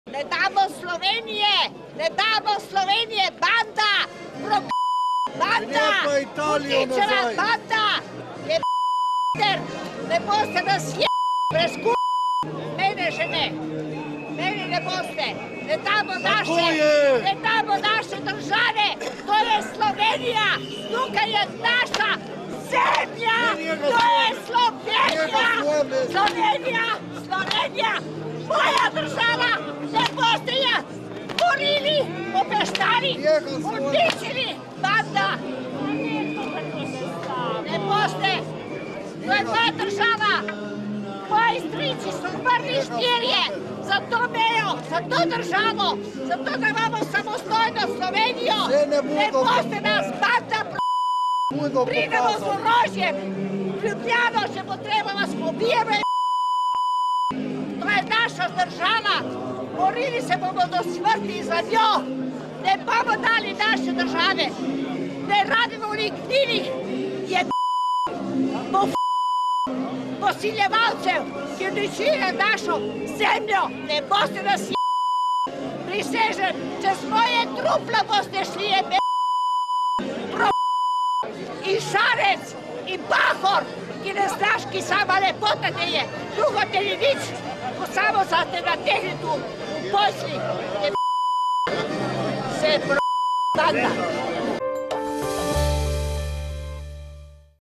"To je Slovenija! Tukaj je naša zemlja!"
udarjanje z zastavo :))
Sobotni protest v Jelšanah, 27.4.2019